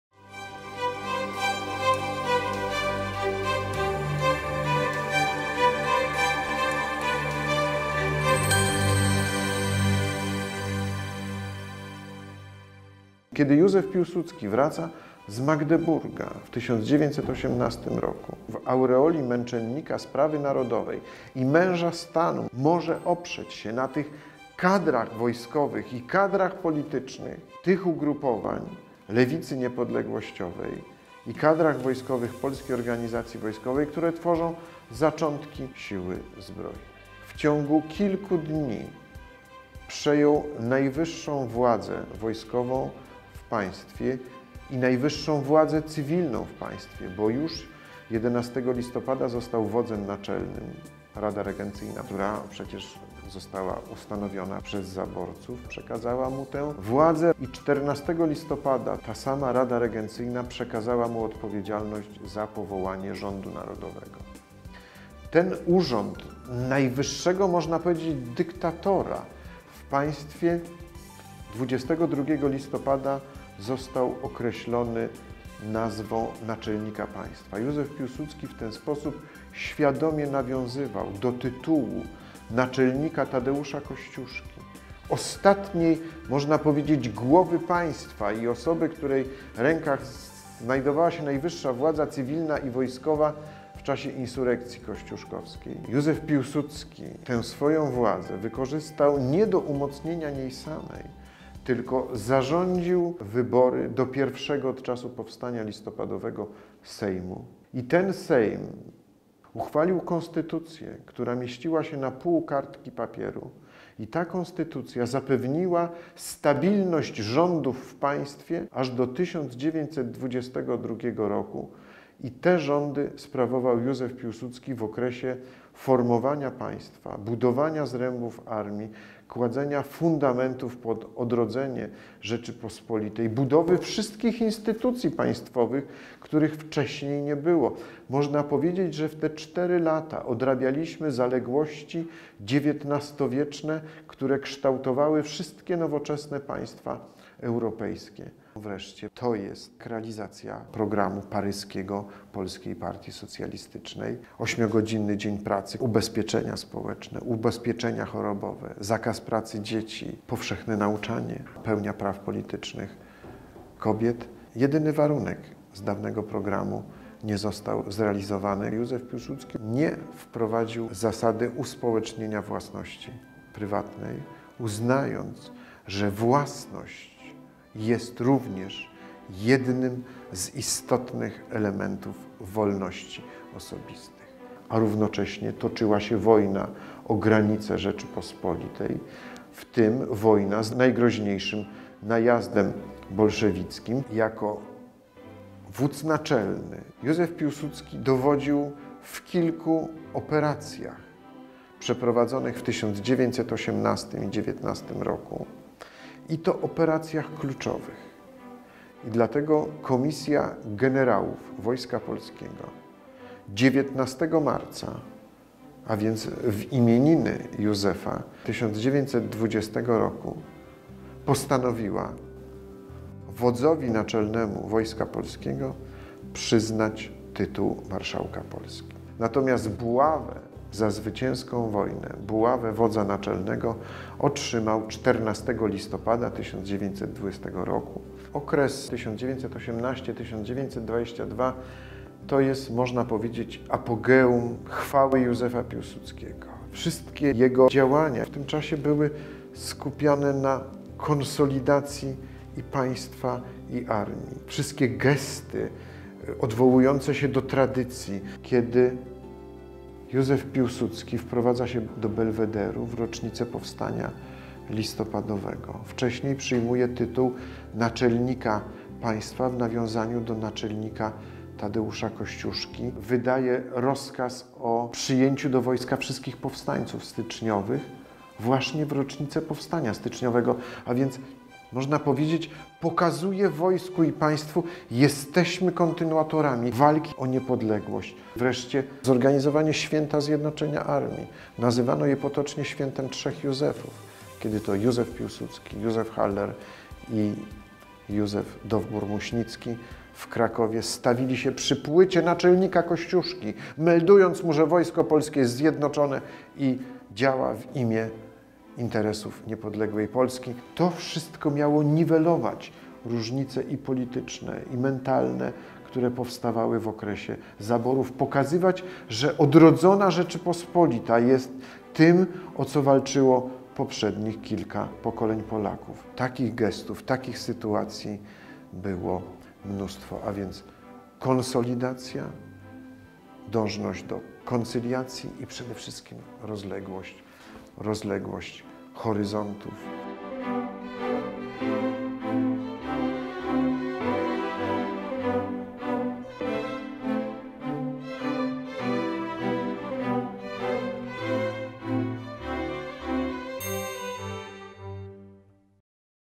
Test Wiedzy o Józefie Piłsudskim (część IV: Naczelnik 1918-1926)
Test Wiedzy o Józefie Piłsudskim to projekt realizowany przez Fundację Rodziny Józefa Piłsudskiego ze wsparciem Banku Zachodniego WBK oraz Muzeum Józefa Piłsudskiego w Sulejówku. Jego celem jest popularyzacja wiedzy o Pierwszym Marszałku Polski oraz przybliżenie jego postaci młodzieży. Test kierowany jest w szczególności do uczniów szkół gimnazjalnych i ponadgimnazjalnych oraz pedagogów. Jako przygotowanie do Testu stworzona została Platforma Wiedzy o Józefie Piłsudskim, na której znajdują się fotografie i artykuły dotyczące Marszałka i jego czasów, umożliwiające przygotowanie się do Testu.